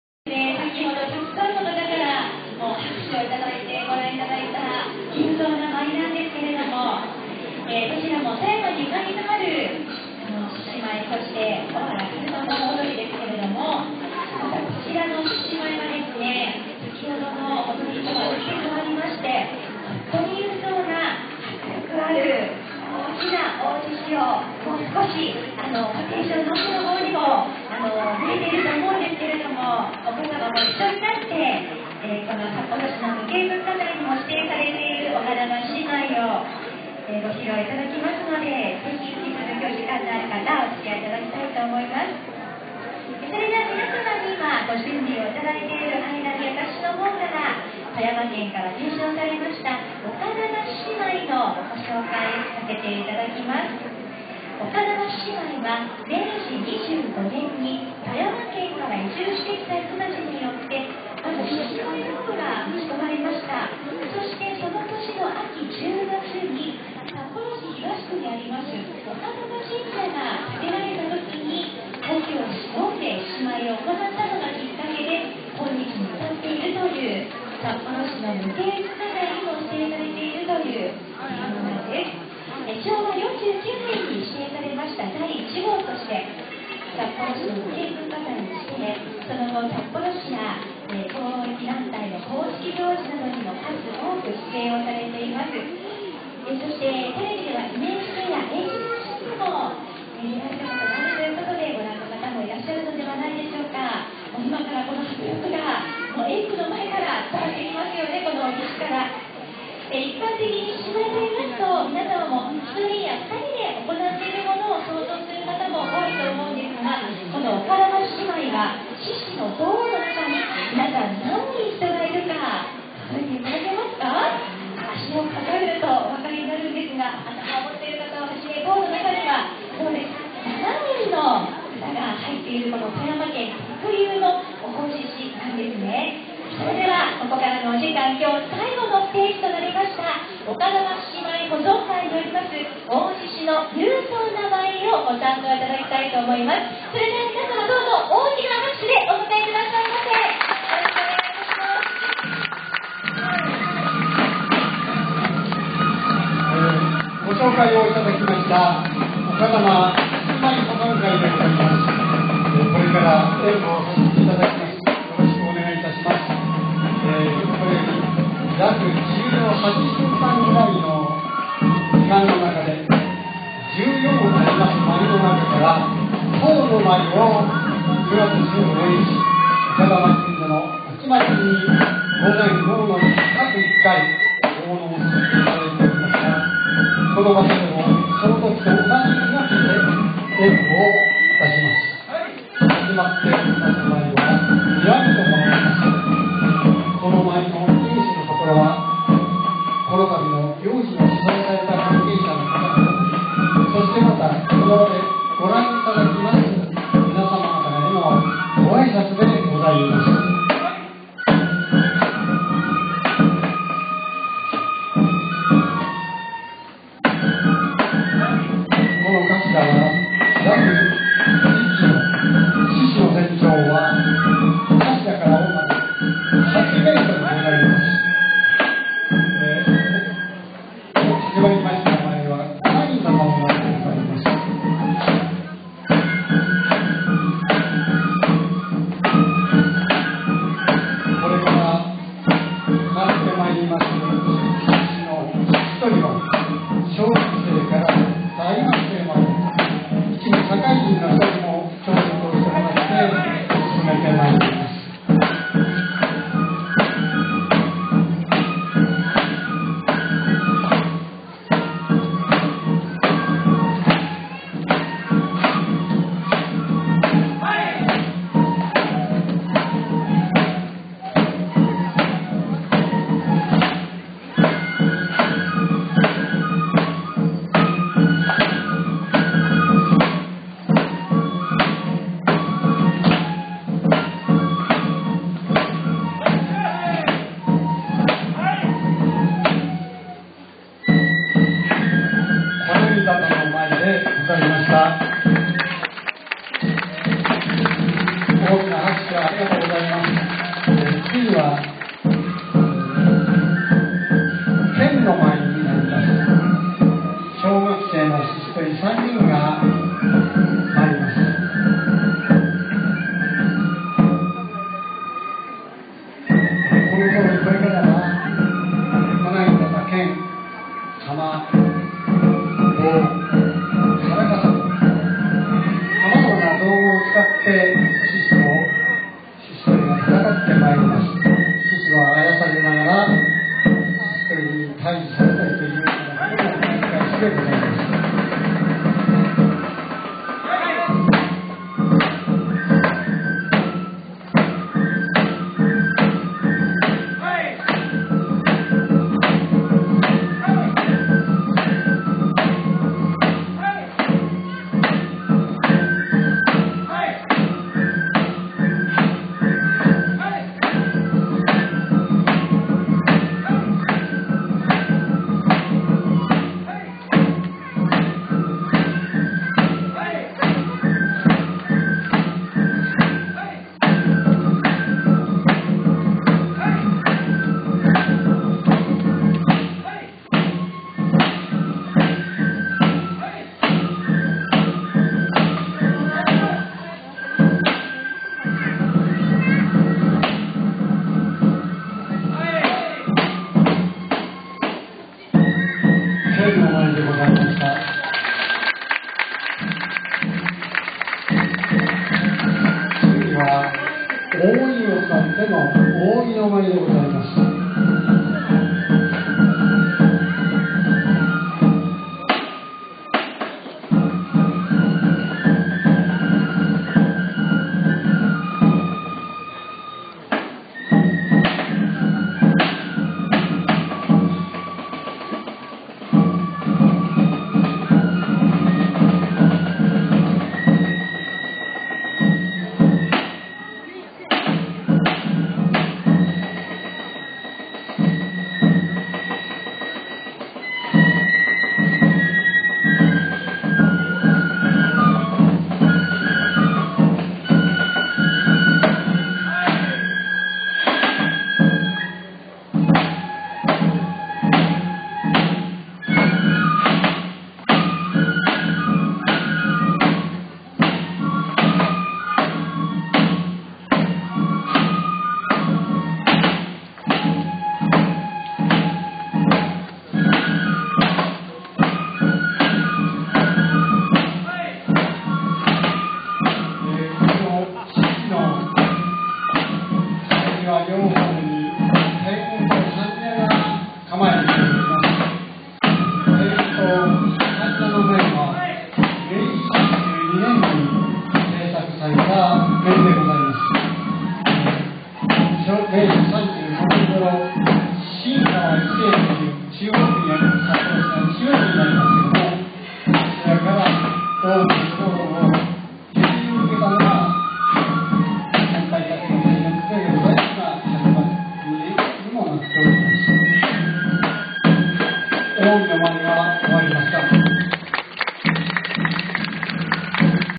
札幌市無形文化財 丘珠獅子舞（１）20110709
札幌駅前通地下歩行空間を歩いているとPRイベントが行われていたので足を止めて撮影しました

iPhoneから録画されました 11/07/09 14:27 JST

丘珠獅子舞（北海道文化遺産データベース）

丘珠獅子舞（地域文化資産ポータル）
with the Ustream Mobile App